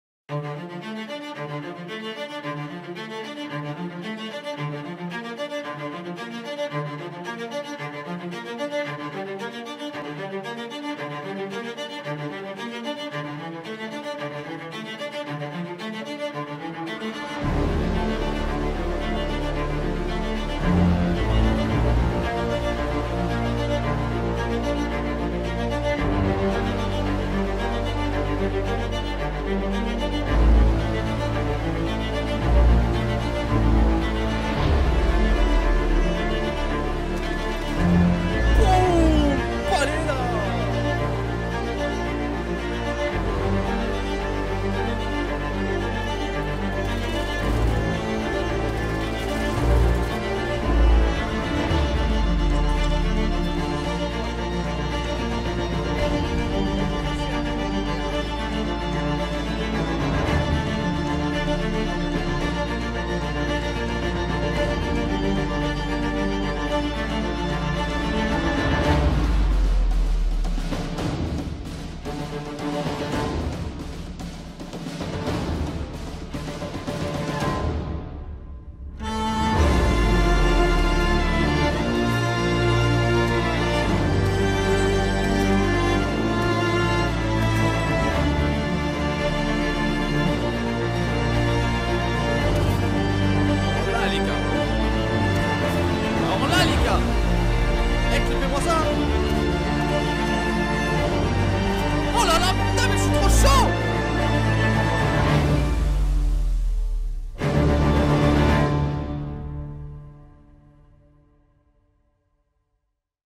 Un nouveau départ - Bande d'annonce - Deapforce/Zozo
Reprise des vidéos avec cette petite bande d'annonce. On se lance dans le streaming et Youtube a 100% donc n'hésitez pas à venir découvrir tout ça avec nous :D